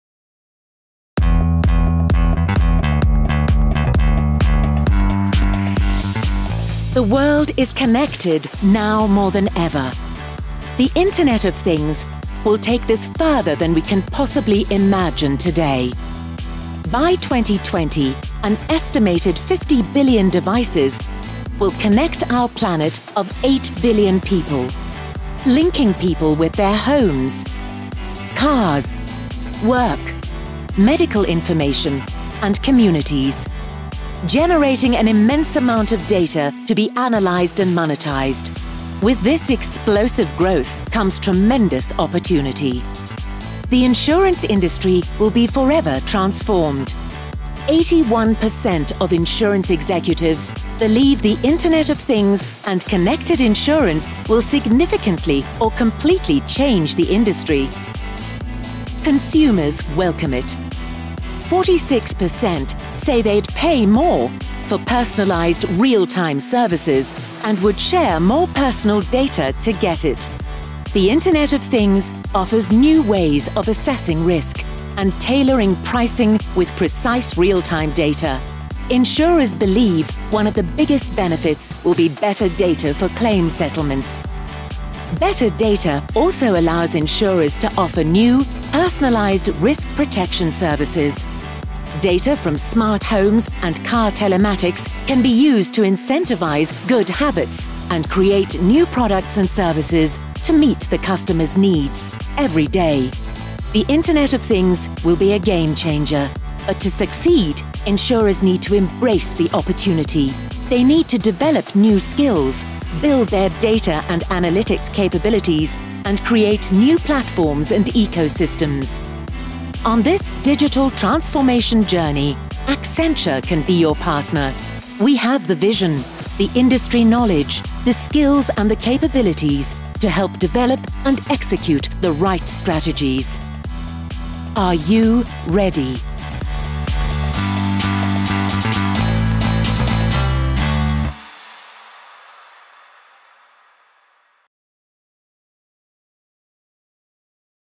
Insurance and the Internet of Things
The Internet of Things will transform the way we live and work. People will have the capability to be constantly connected to their homes, cars, medical records, and work. This will generate a huge amount of data and offers insurers opportunities to provide personalized risk protection services, safe driver incentives, and more. Accenture has the vision, industry knowledge, skills and capabilities to help insurance organizations develop and execute the right IoT strategies.